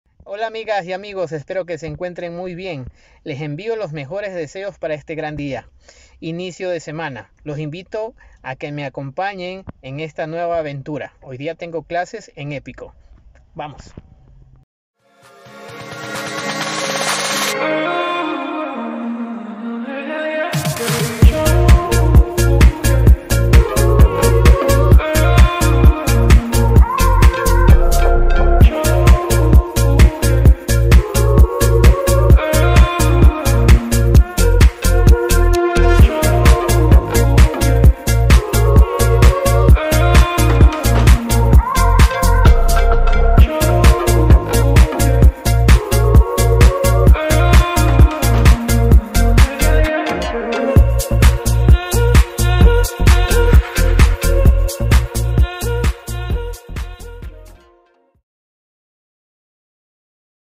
Nueva aventura 🏆🥇🏋️‍♀️
¡Hola a todos! Les tengo una gran noticia, estoy emocionado de anunciar que estoy comenzando una nueva aventura, ¡clases en Épico! 🎉🤩

En estas clases, podrán aprender como fortalecer tu emprendimiento, es un ambiente único y motivador. Asesorados por grandes profesionales como: Sarah, Francisco, Liubinsky 💪🏼🎉🤩

¡Es una experiencia épica! 💪🏼🌟

Si quieres unirte a esta aventura, realízalo mediante @epicogye donde podrás encontrar toda la información necesaria de Épico.

¡Te espero! 🙌🏼